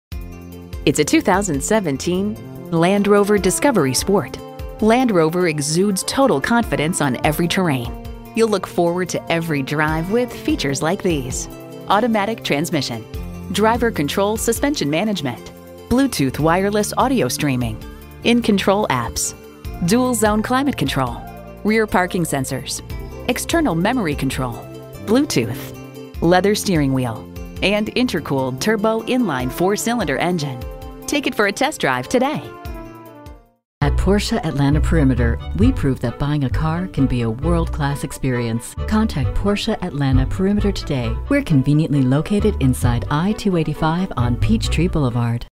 Used 2017 Land Rover Discovery Sport Atlanta, GA PP17208B - SOLD
Year: 2017
Make: Land Rover
Model: Discovery Sport
Trim: SE
Engine: 2.0L I4 Turbocharged
Transmission: 9-Speed Automatic
Color: Black
Interior: Ebony/Ebony
Mileage: 46387
Stock #: PP17208B
VIN: SALCP2BG2HH688295
3rd Row PackagebrClimate Comfort PackagebrAudio Option PackbrAmbience Lighting 2brCD NavigationbrbrWe look forward to hearing from you. Advertised price includes $699 Dealer Service Fee.

Address:
4006 Carver Drive